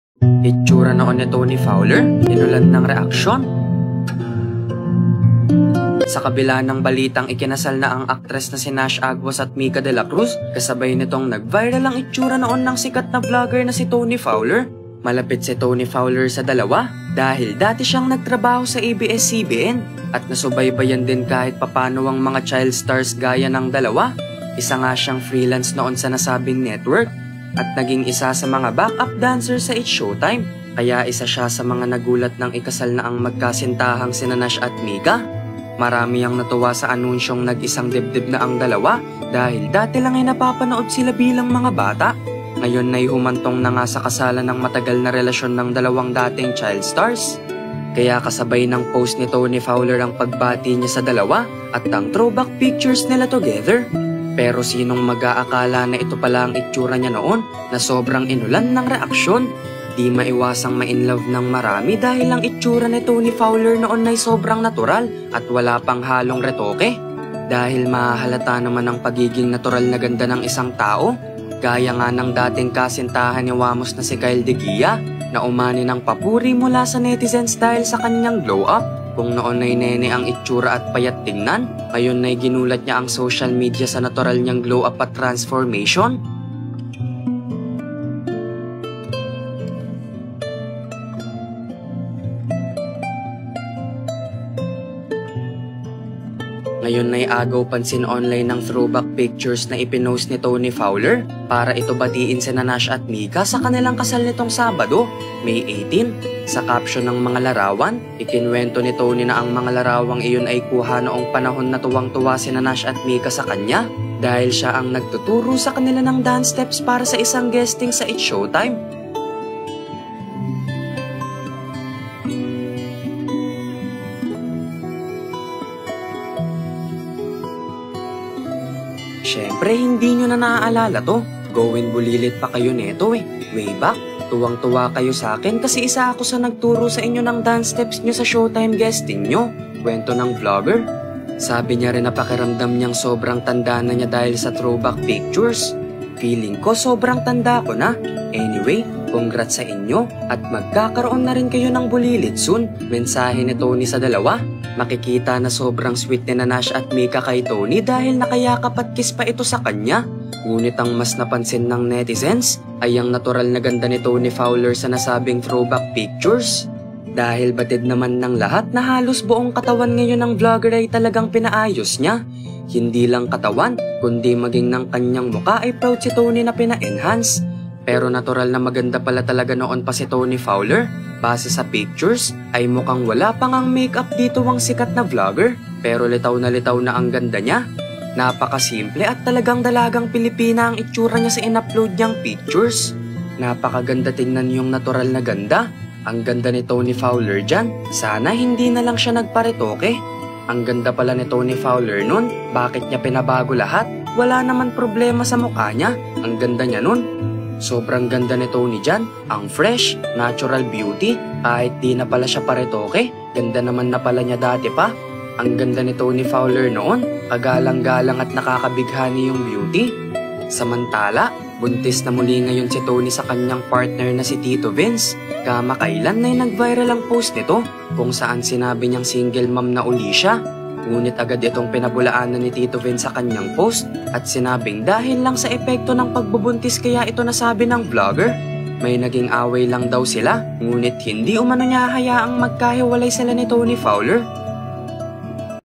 Itsura noon ni Toni Fowler, inulan ng reaksyon
Kasabay ng pagbati ni Toni Fowler sa bagong kasal na sina Nash at Mika ay naging agaw-pansin rin ang itsura niya noon!